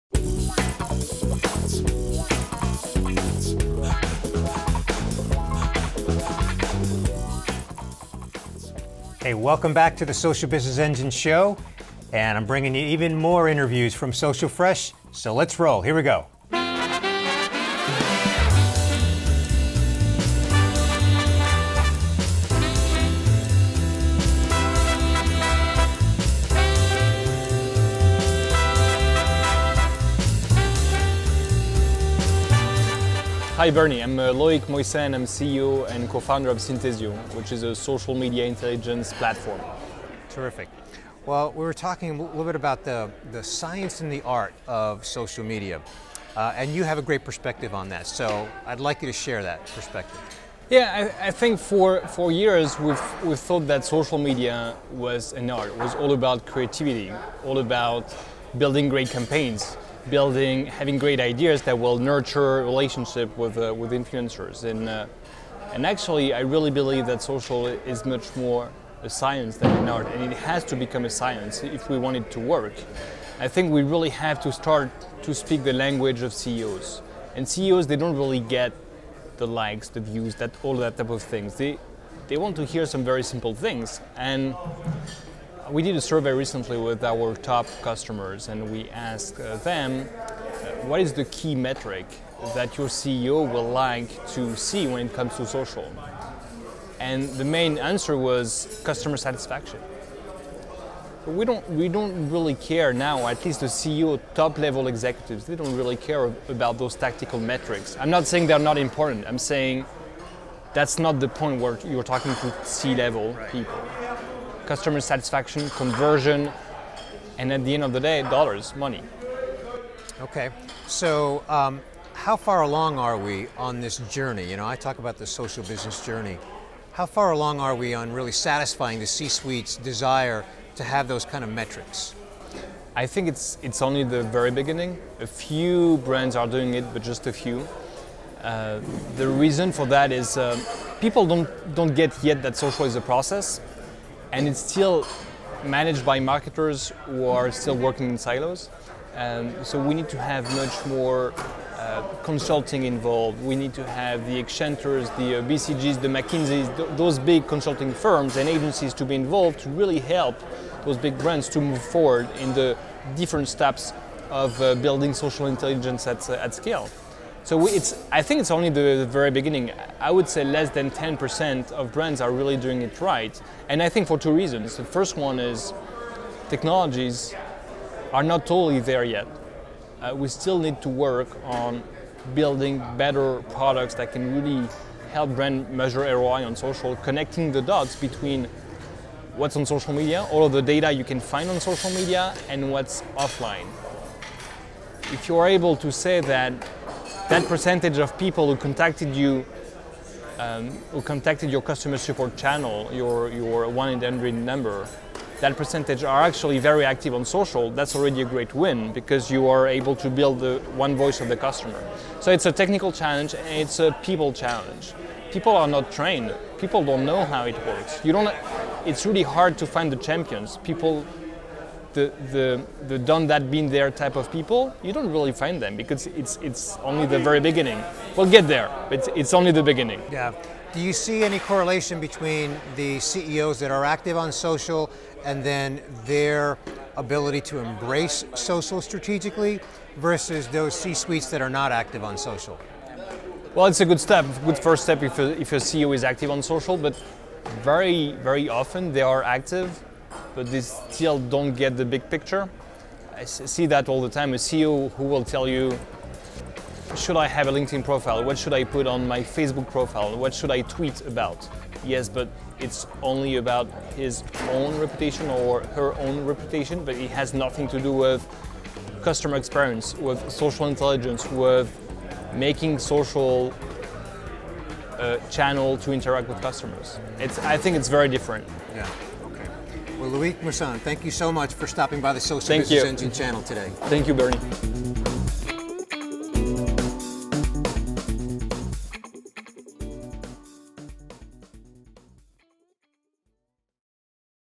The Science of Social Media with Loic Moisand
Filmed on location at Social Fresh East 2014, Bernie Borges talked with Loic Moisand of Synthesio about the science of social media and the metrics the C-Suite really cares about.